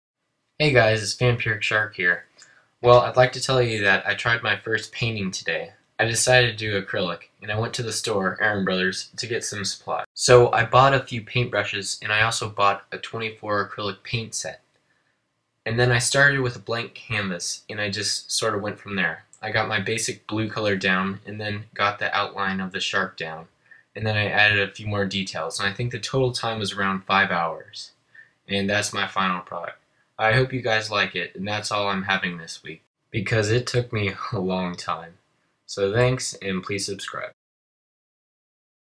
My First Painting: Great White Shark
Hey guys toady I painted for the very first time.  I decided to go with a Great White Shark hunting a few seals.  Sorry that this is not a time lapse video but it took me a very long time to paint this.  This was my first time ever painting and I have no experience.  I have never taken a painting class I just applied my drawing skills to the canvas.  As I said in my video, I had some cash laying around so I drove to my local art store, Aaron Brothers and picked up some acrylics, brushes, and a canvas.  I search the many internets for a suitable image of a shark.  Once I found a cool picture, I began to paint.  I laid down a basic blue coat and then painted the basic shape of the shark.  I added more details and shading.